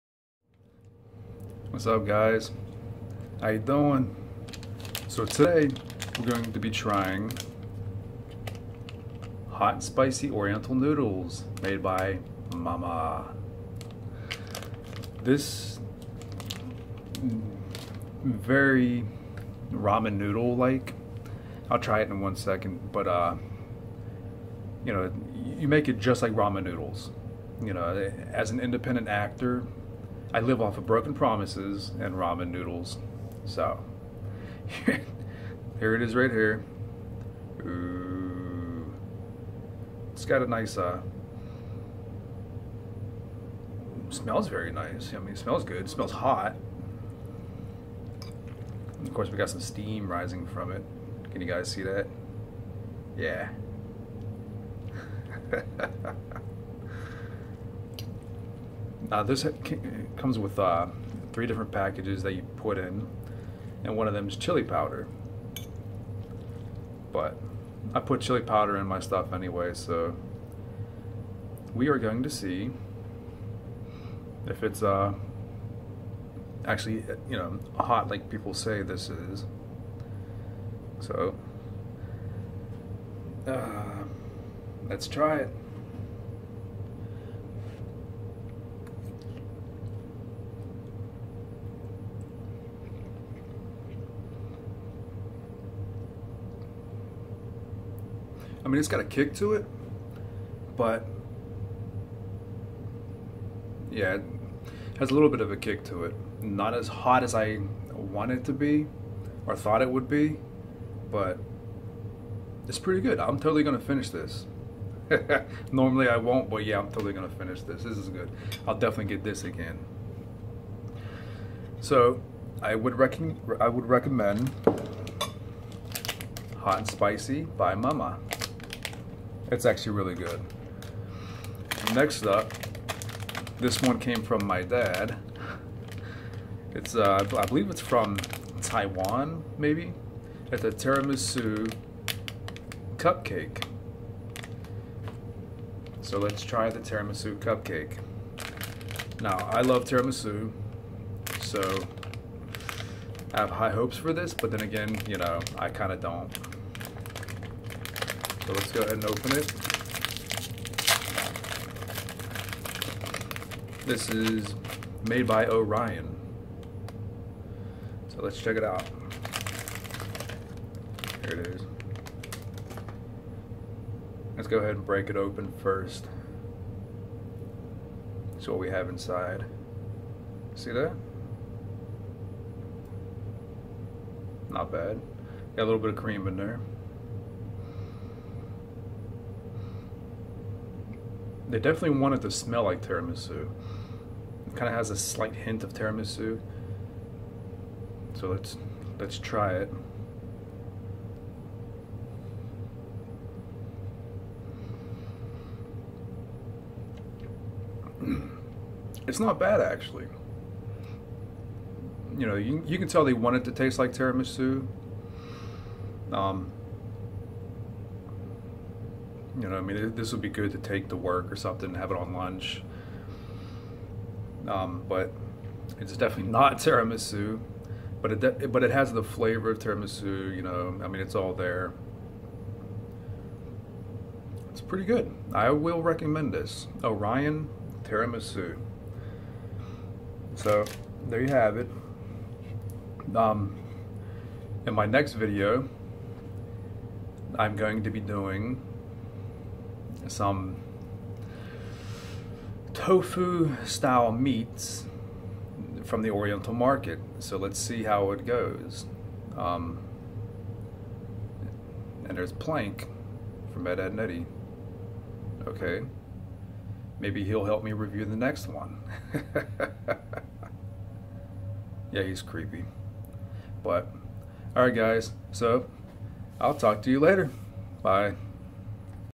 I try... Mama's 'Hot & Spicy' noodles and Orion's 'Tiramisu Cupcake'.
I get to try Hot & Spicy noodles by Mama as well as a Tiramisu Cupcake by Orion.